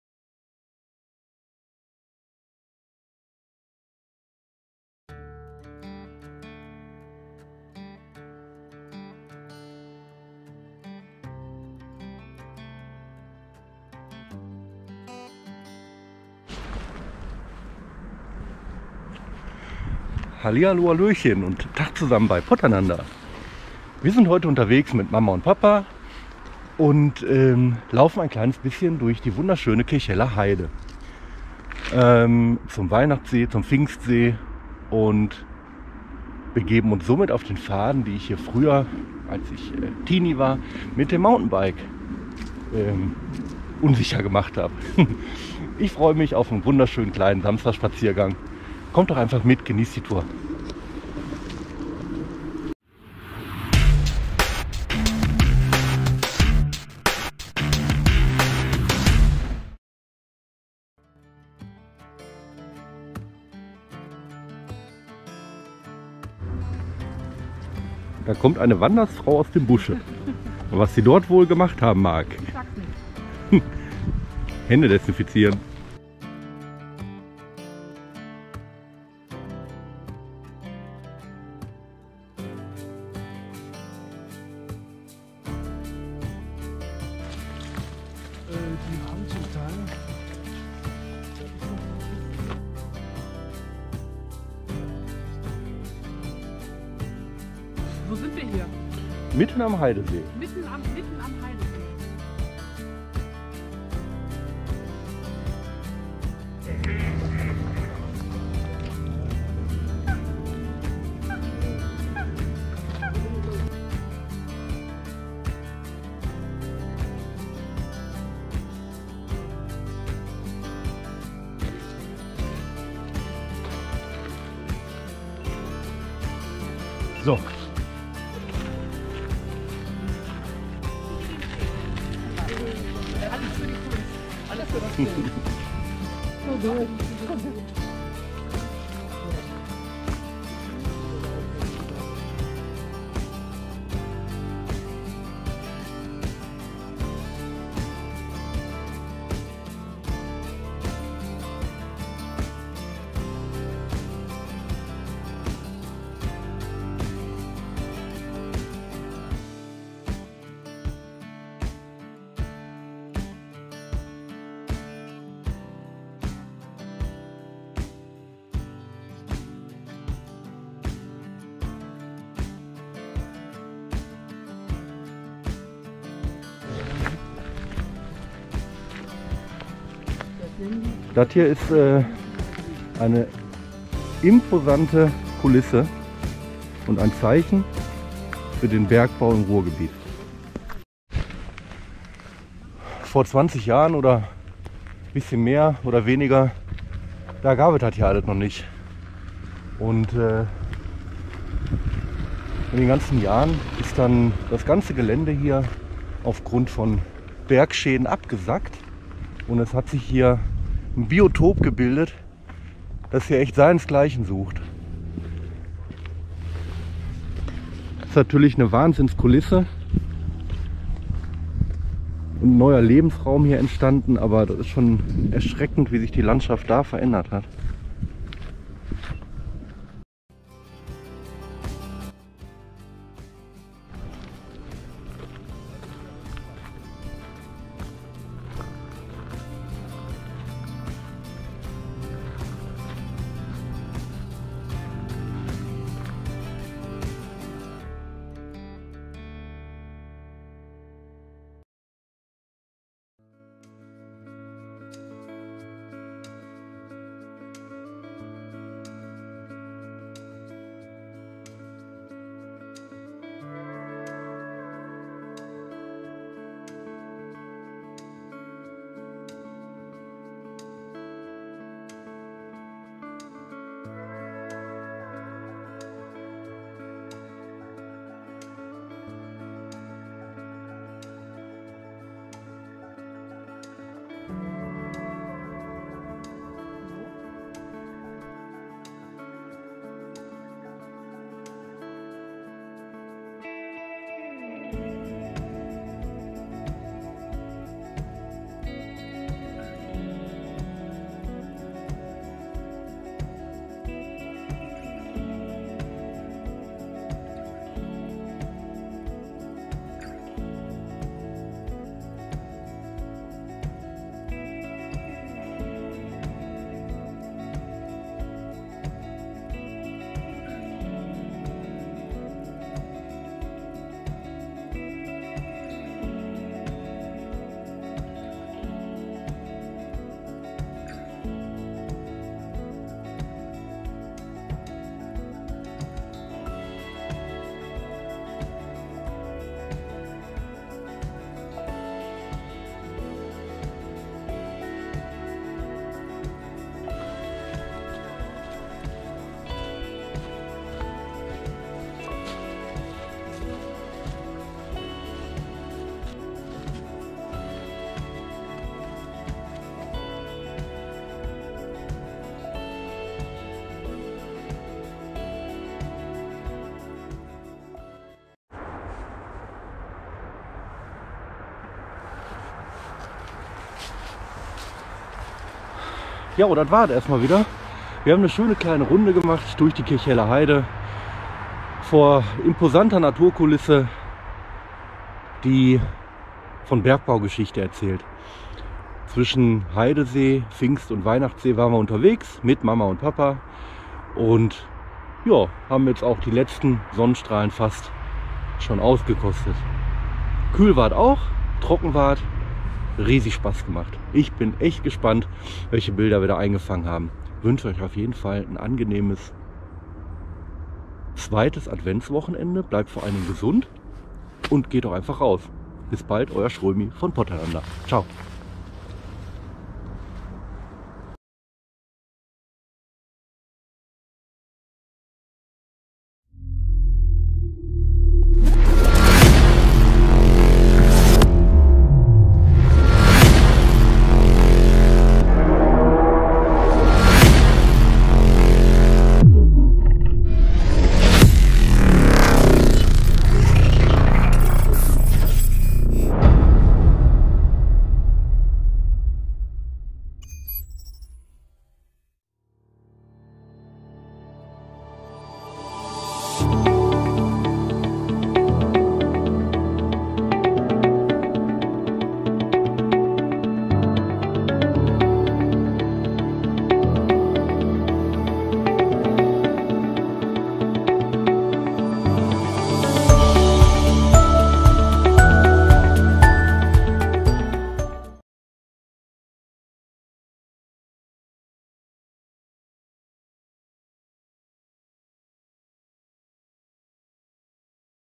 3-Seen-Runde: Kirchheller Heide
POTT.heide

3-Seen-Runde in der Kirchheller Heide: Zwischen Heidesee, Pfingstsee und Weihnachtssee

Im POTT isset am Weihnachten meist nass
...oft kann man sogar postulieren, dass Weihnachten auch mal wettermäßig ins Wasser fällt...WENN der Weihnachtsmann nicht sowieso gerade baden geht...

Und der Santa C. macht dat dann bekanntlich bei uns im POTT.

Genauer: In Bottrop-Kirchhellen.

Glaubt Ihr nicht?

Is' aber so!!

Der Weihnachtsmann braucht ja auch mal Wellness, Spa & Co.

Die Adventszeit ist aber auch immer so stressig...und deshalb pflegt der füllige Mann im roten Samtmantel seine Seele eben in der Kirchheller Heide. Dat kann man da amber auch besonders gut!

Ihr glaubt mir immer noch nich', ne?

Ja, wat meint Ihr denn, warum es denn in dem Teil des Naturparks Hohe Mark extra einen WEIHNACHTSSEE gibt? - Und warum der wohl so heisst?

Bestimmt nicht, weil es hier so hübsch weihnachtlich- dekoriert aussieht...

Näh, im ernst, getz:

Der Weihnachtssee ist ein Naturschutzgebiet in Bottrop und hat seinen Namen daher, weil er an Weihnachten entstanden ist. (Jetzt aber wirklich!)

Ursprünglich schlängelte sich hier der Schwarzbach (es gibt auch noch den Rotbach) durch den Auenwald.

Und typisch für unsere Region, wo der Bergbau Einfluss auf die Veränderungen in der Landschaft hat, so ist auch dieser See durch Bergsenkungen entstanden. Die Bergsenkungen haben durch den Kohleabbau derartige Spuren hinterlassen, dass das Bachwasser des Schwarzbachs in eine Mulde versunken ist und sich immer weiter aufgestaut hat, bis schlussendlich ein See entstanden ist. Das ganze war dann zu Weihnachten ca. 2001~ 2003. Und das stimmt jetzt aber WIRKLICH!

...Tja und seitdem kommt da immer der alte, dicke, bärtige Mann mit dem Schlitten und dem Sack an und zieht ein paar Bahnen...

In der Tat ist hier im laufe der Jahre eine total neue Landschaft entstanden, da man die natürliche Entwicklung zugelassen hat. Viele Tiere haben sich hier angesiedelt, u.a. sogar der Eisvogel.

...Es ist entspannend, erholsam, einfach traumhaft und idyllisch. - Der Weihnachtsmann hat halt einen guten Geschmack! Und so ist der Weihnachtssee und die Kirchheller Heide ein einzigartiges Naherholungsgebiet und lockt viele Spaziergänger an, die vor allem Ruhe und Natur suchen und vor allem finden.

Mit 13 Jahren habe ich hier angefangen meine Runden mit dem Fahrrad zu ziehen und ich erinnere mich an eine völlig andere Landschaft. Gerade hier sind Senkungen und Steigungen entstanden, die es damals noch nicht gab. Heute im Vergleich zu damals ist der Wald streckenweise nicht wiederzuerkennen...aber nicht ein bisschen weniger attraktiv!

Mein Tipp für Euch:

MACHT ES EINFACH WIE DER WEIHNACHTSMANN!

...kommt nach Bottrop in die Kirchheller Heide und genießt die Naturpark Hohe Mark insbesondere in der stressigen Advents-/ Vorweihnachtszeit...

Und vielleicht ist der Weihnachtssee ja für Euch dieses Jahr ein Ziel für den Verdauungsspaziergang nach dem Festessen?

Habt auf jeden Fall eine besinnliche Zeit!

GLÜCK AUF!

Euer 'Schrömi'.